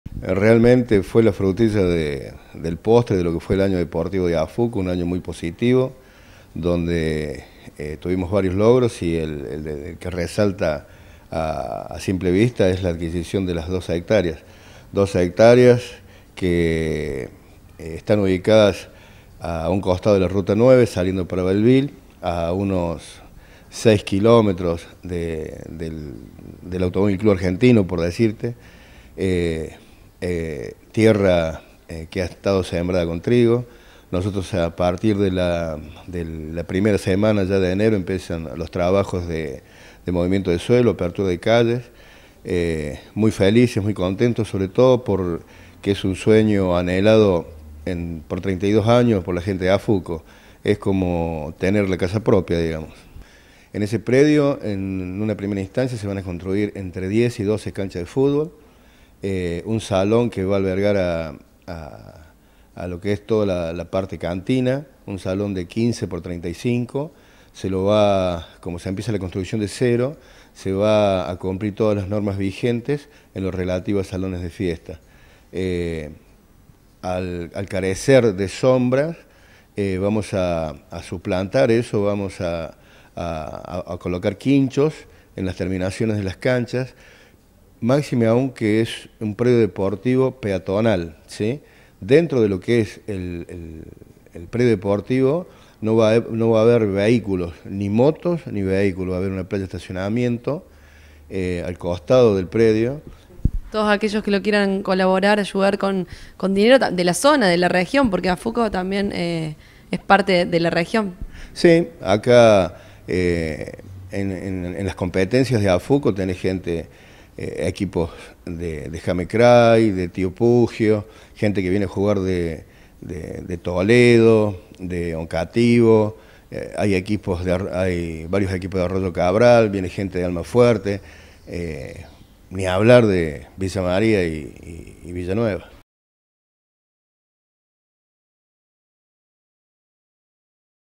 AFUCO: COMIEZAN EL AÑO CON PREDIO PROPIO
La asociación de futbol comercial adquirió un predio de 12 hectáreas al costado de la ruta nacional n° 9, frente al parque industrial donde desde el próximo mes comenzara con la obra de las instalaciones. Proyectan 12 canchas de futbol y un salón de fiestas.